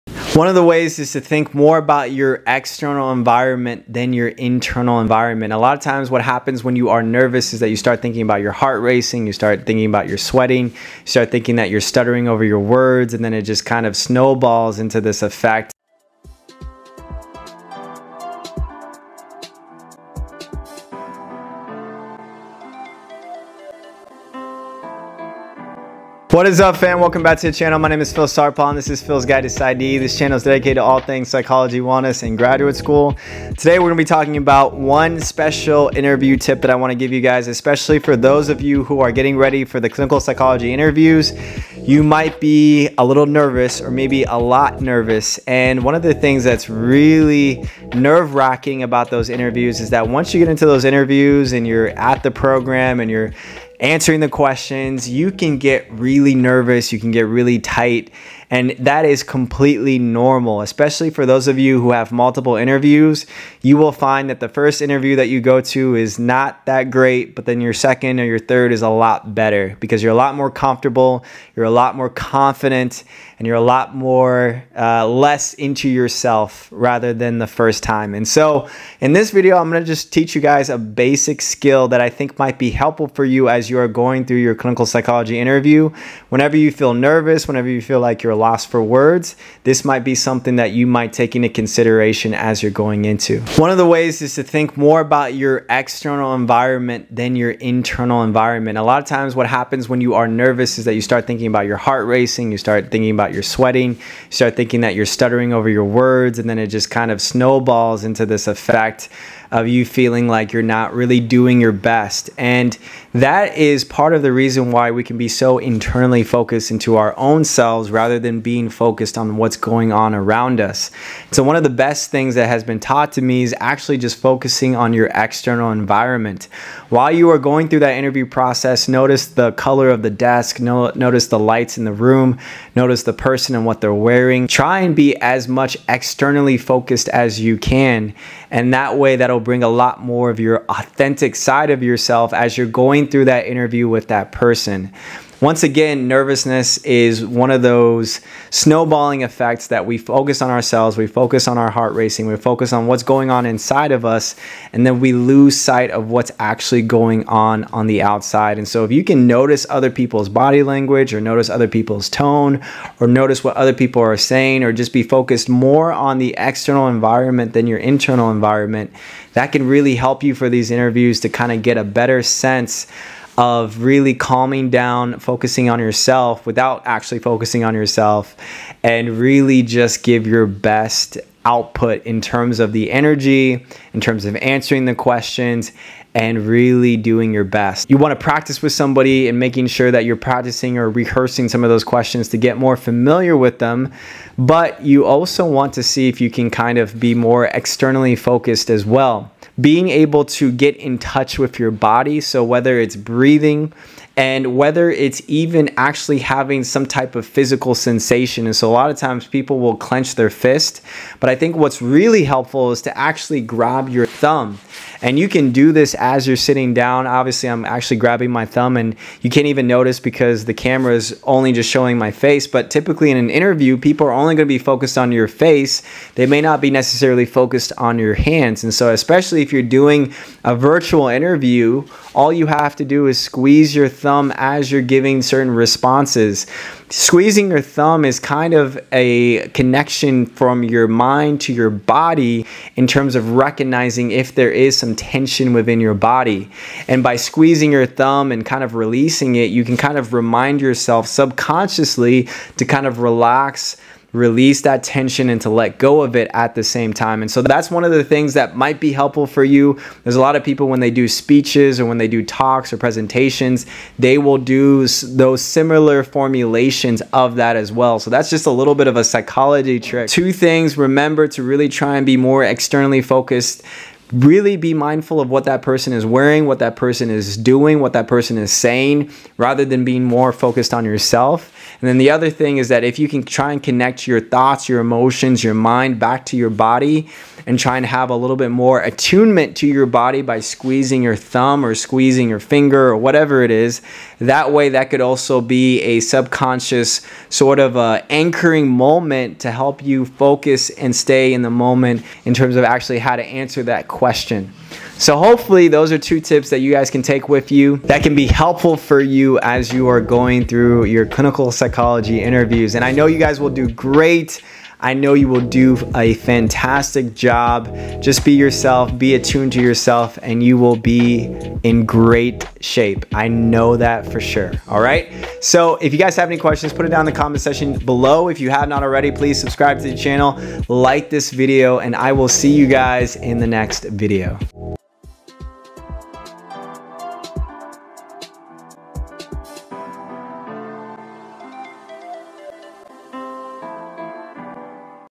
How to Calm Nerves During an Interview | 2 Evidence-Based Tricks | Clinical Psychology
This video is not intended as professional or legal advice. Be sure to seek the services of a professional if you are in need of them.